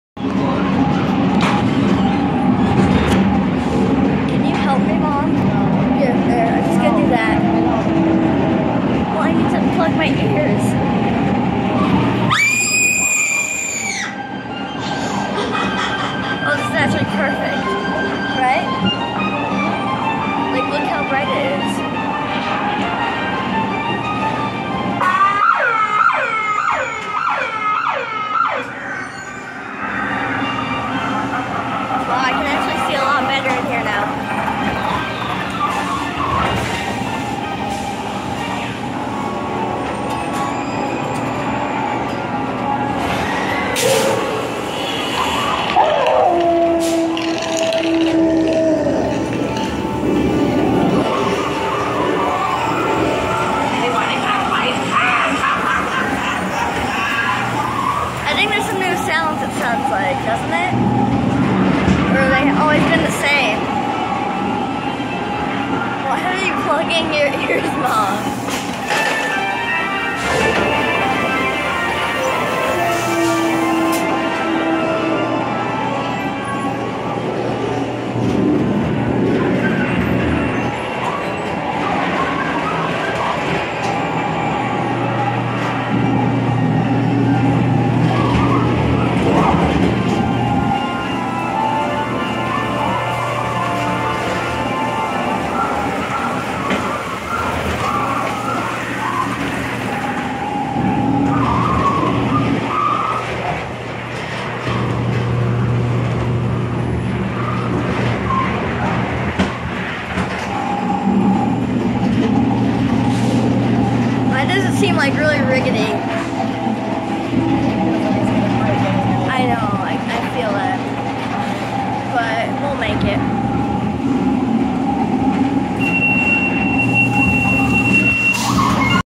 Draculas Castle Lagoon 2018
Draculas Castle is a dimly lit level ground coaster that takes you through a haunted castle with monsters and jumpscares around every corner.